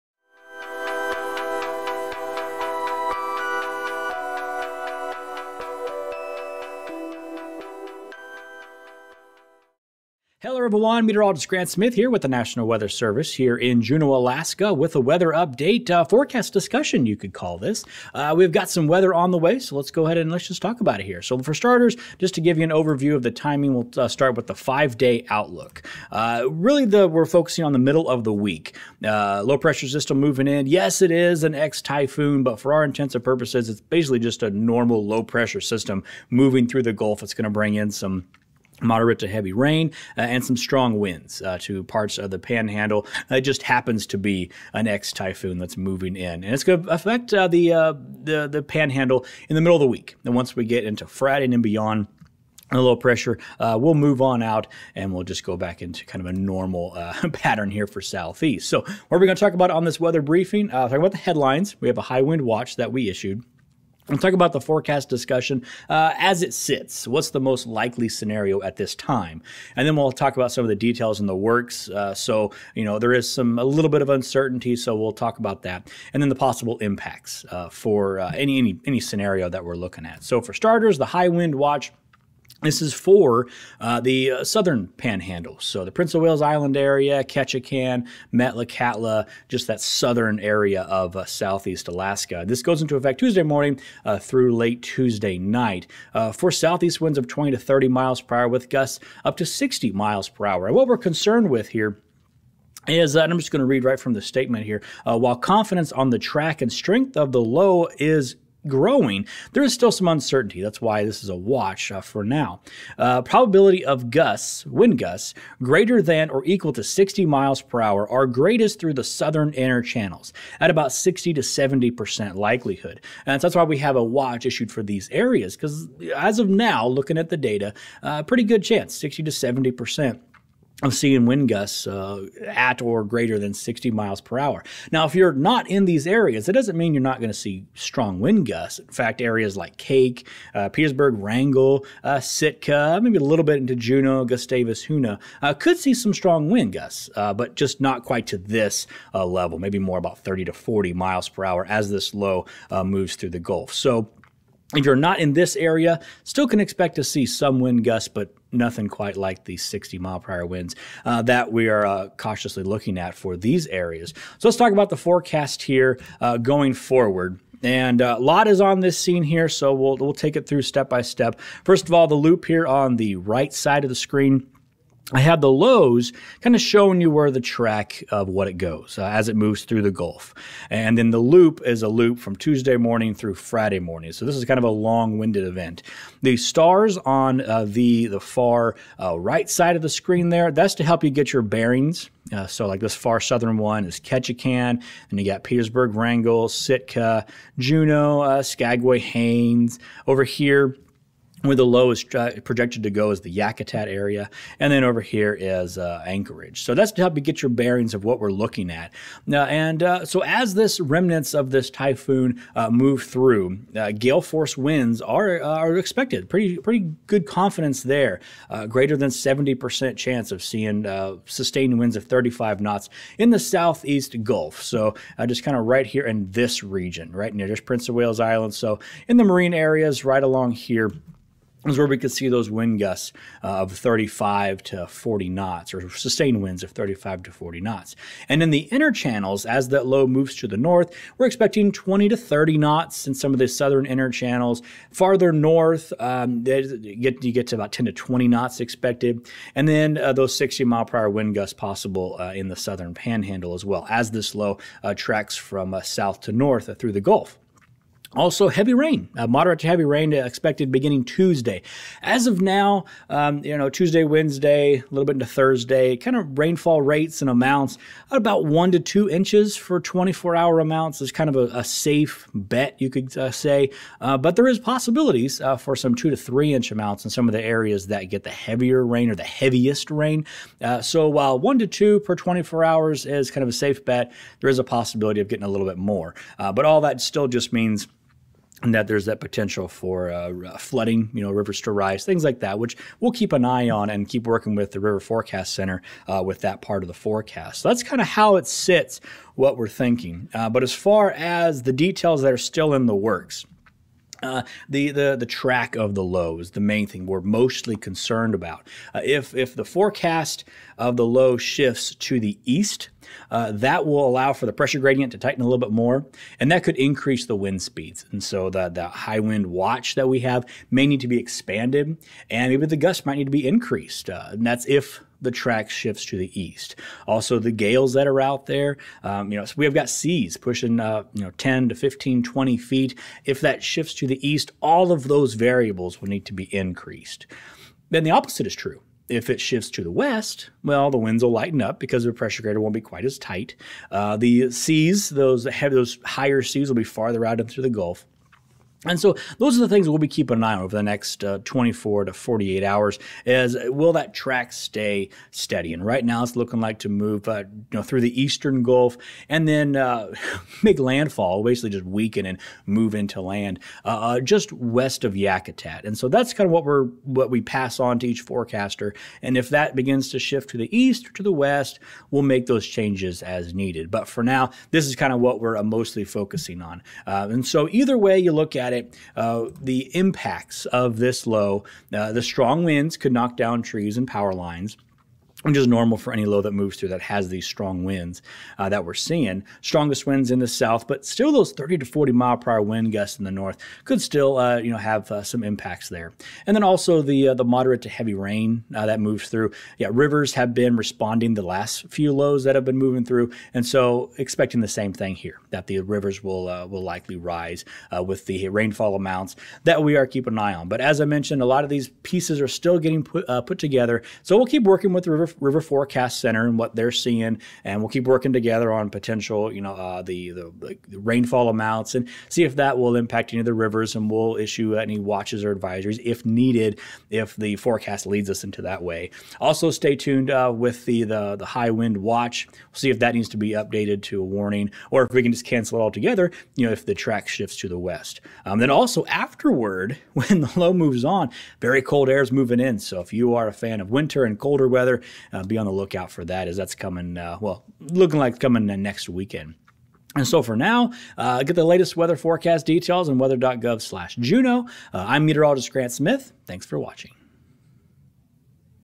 Another Round of Rain and Wind - SE AK Weather Brief October 15, 2023
Remnants of Typhoon Bolaven will track through the gulf, giving the panhandle another round of moderate to heavy rain and strong winds. Meteorologist Grant Smith talks about the forecast details and the forecast pieces that are coming together.